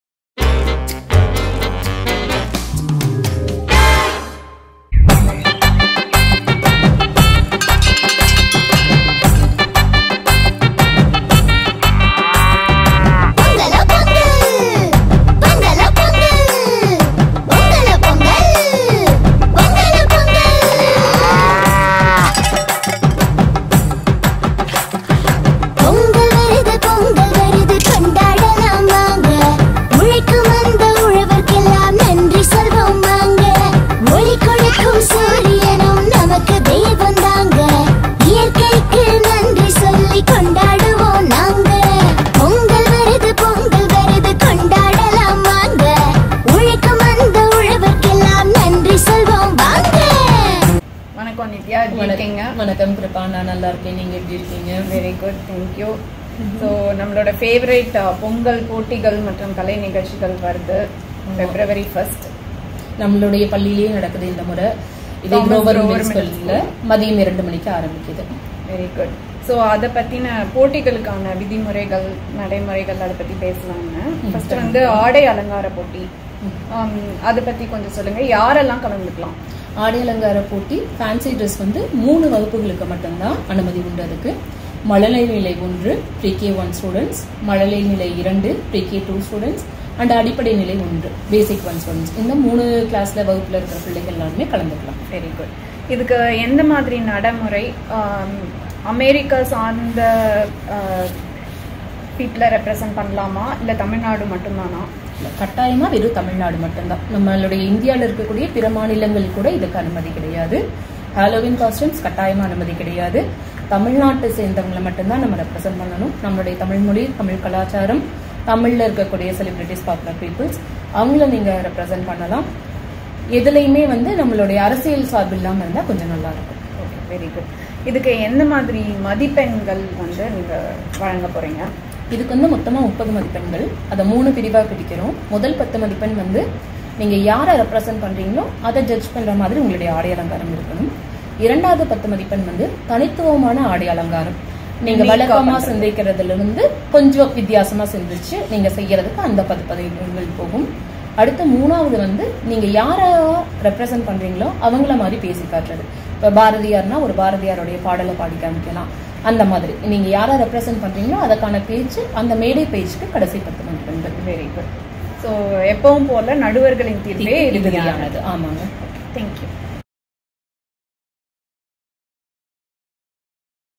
ஆடை அலங்காரப் போட்டி விதிமுறைகள் | பொங்கல் விழா 2020
வள்ளலார் தமிழ்ப் பள்ளி, நியூசெர்சி

Vallalar Tamil School, New Jersey

பொங்கல் விழா 2020 : Vallalar Tamil School Pongal Vizha 2020